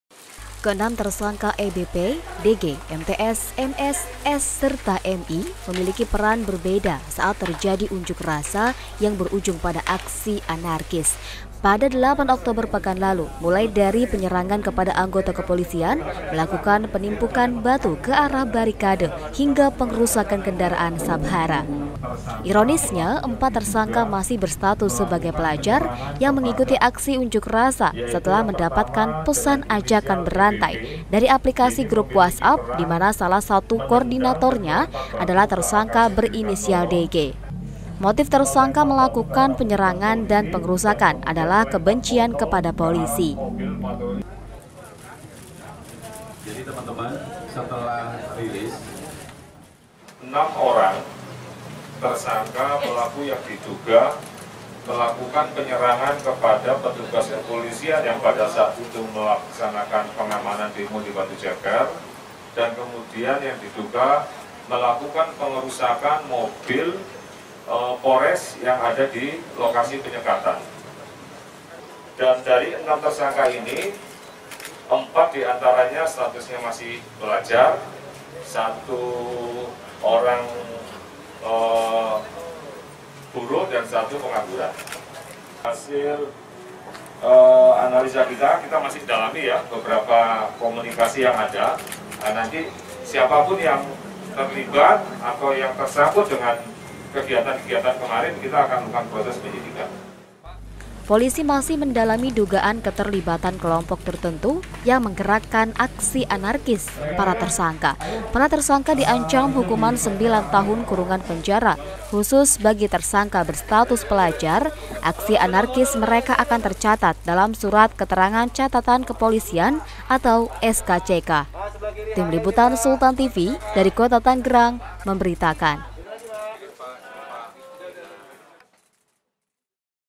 6 Tersangka Aksi Unjuk Rasa Rusuh di Batuceper - Berita Sultan
Kepolisian resot metro Tangerang Kota, menangkap enam tersangka penyerangan anggota kepolisian, dan pengerusakan kendaraan saat kerusuhan pada unjuk rasa penolakan undang-undang omnibuslaw di Daan Mogot, Batu Ceper, Kota Tangerang, pada 8 Oktober lalu.